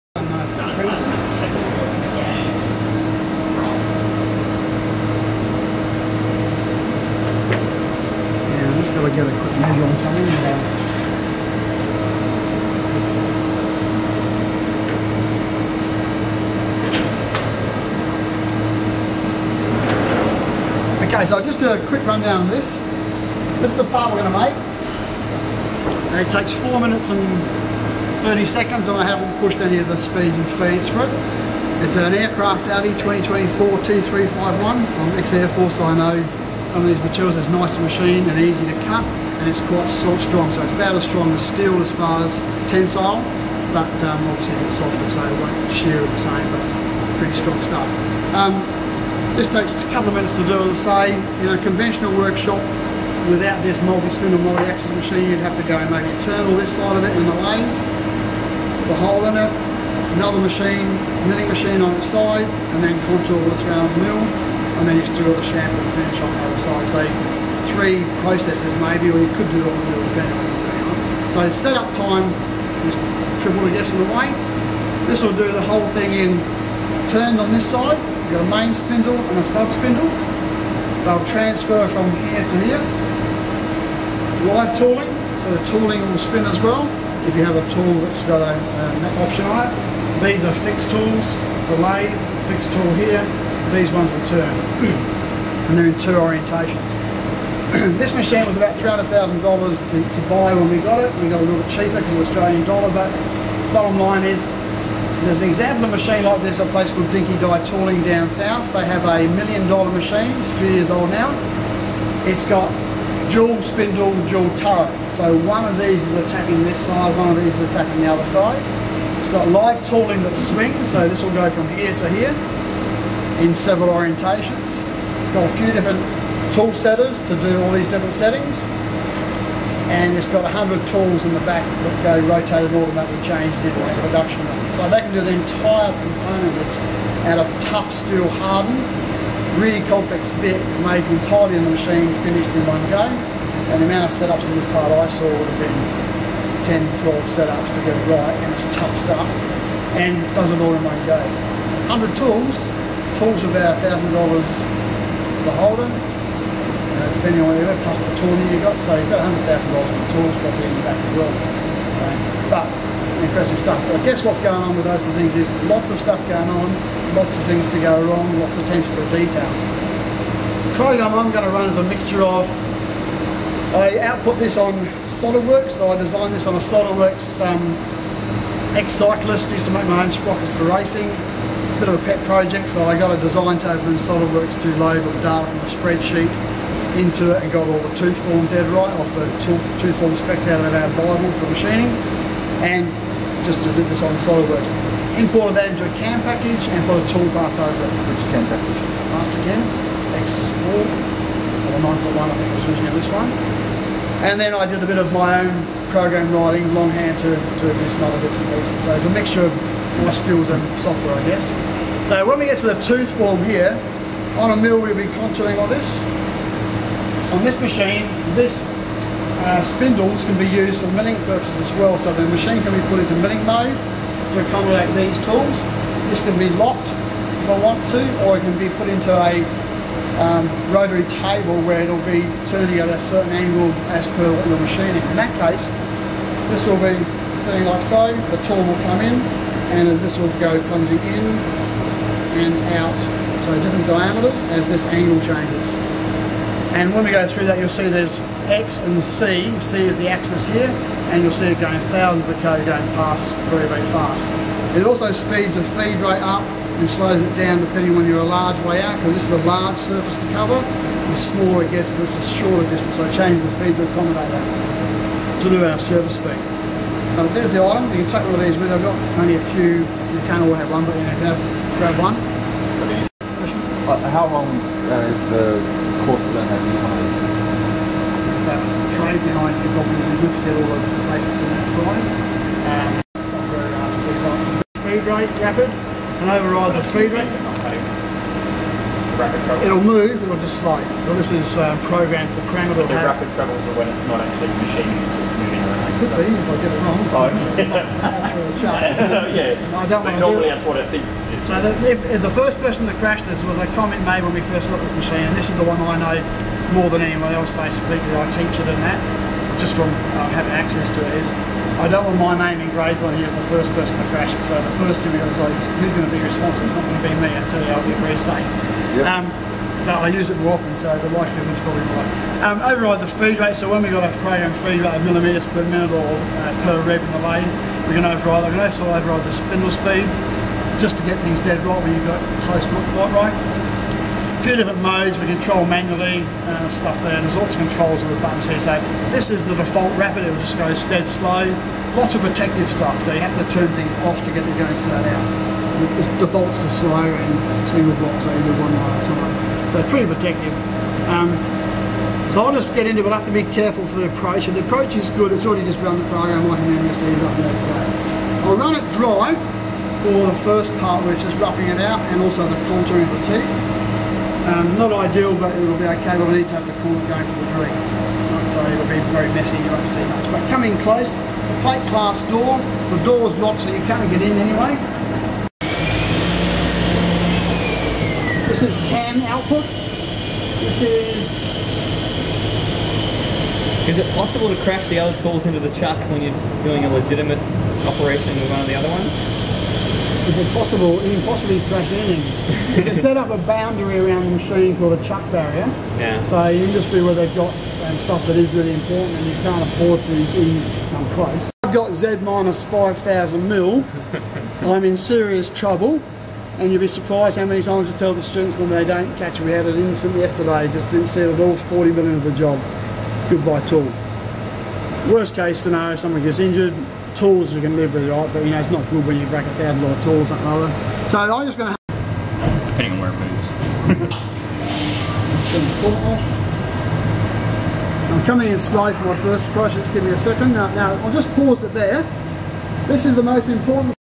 5 axis CNC machining Part 1
Demonstration of a 5 axis CNC milling machine at Regency Park TAFE.